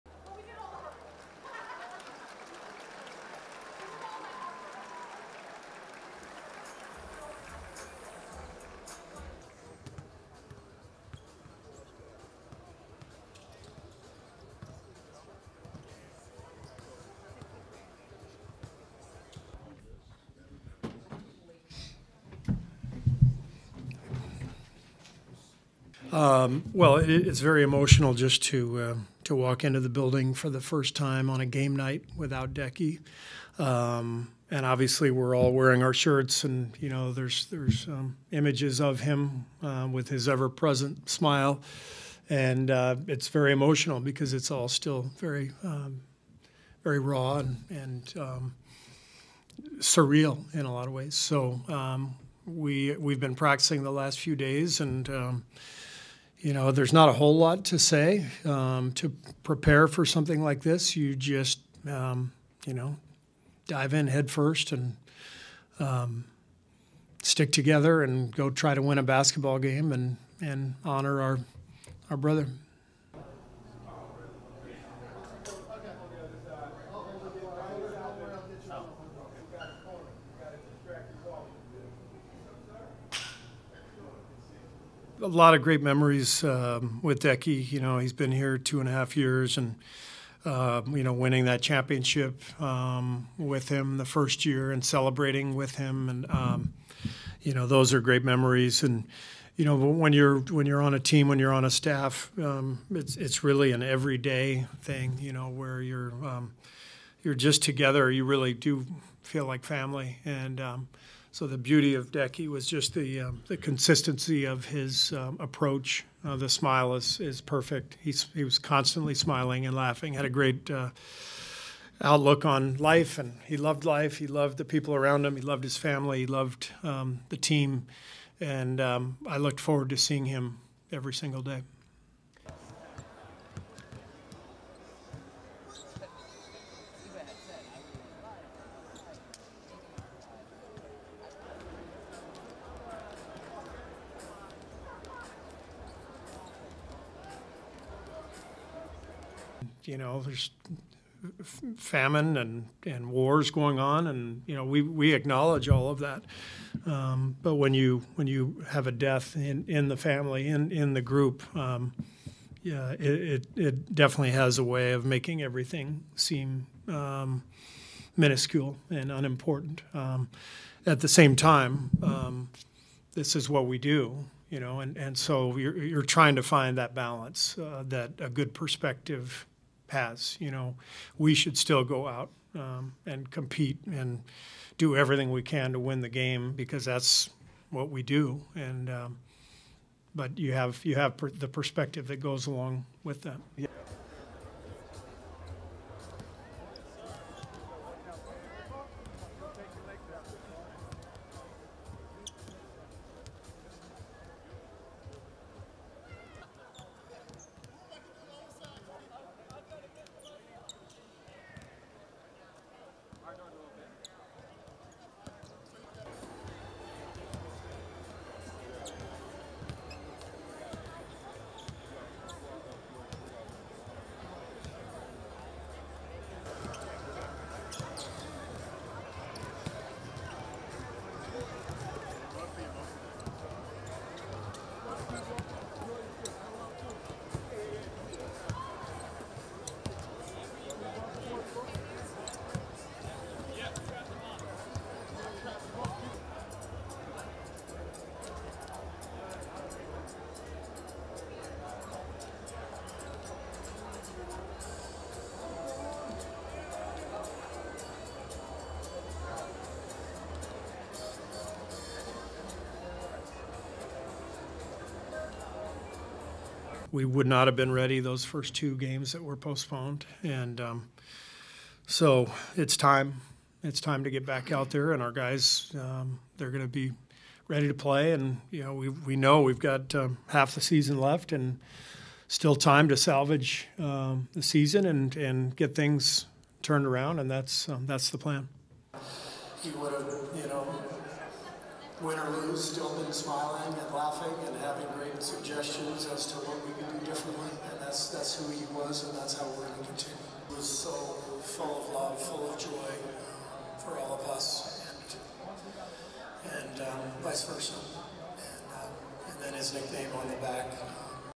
Grieving Warriors play first game after death of assistant coach Dejan Milojevic.
An emotional Golden State Warriors team played their first game after the shocking death of assistant coach Dejan Milojevic.

Following an extended break to grieve, postponing two games, the Warriors took on the Atlanta Hawks on their home court in San Francisco Wednesday night.

Milojevic died one week ago in Salt Lake City at age 46 after suffering a heart attack at a team dinner a night earlier.

Warriors Head Coach Steve Kerr estimated four or five players were at the restaurant and he was there along with other coaches and staff.

He praised vice president of player health and performance Rick Celebrini for his poise, care and professionalism through the ordeal.

The Warriors made counseling services available to players and staff.

Before tipoff, the Warriors wore black “BRATE” t-shirts with Milojevic's initials DJ inside a heart.

Brate means brother and was what the coach called everybody on the Warriors.

Milojevic's initials were also branded on the Warriors home court and will remain there for the rest of the season.

A video paying tribute to Milojevic was played on the big screen before tipoff.